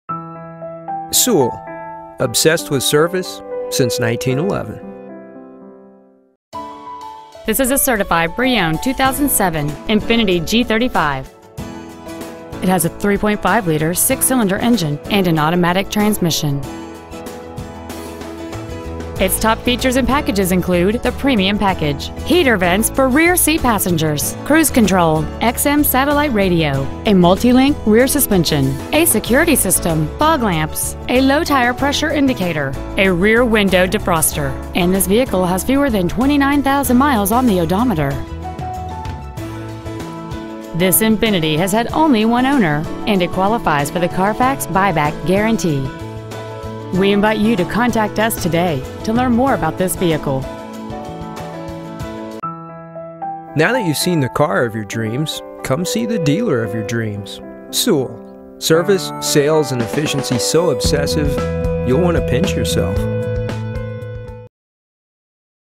2007 Infiniti G35 Sedan Certified Houston TX 77090
We are proud to present this 2007 Infiniti G35 Sedan.

 Year: 2007
 Make: Infiniti
 Model: G35 Sedan
 Engine: 3.5L DOHC 24-valve V6 aluminum engine
 Trans.: Automatic
 Exterior: Other
 Miles: 28,432
 Interior: Other

 Preowned 2007 Infiniti G35 Sedan Houston TX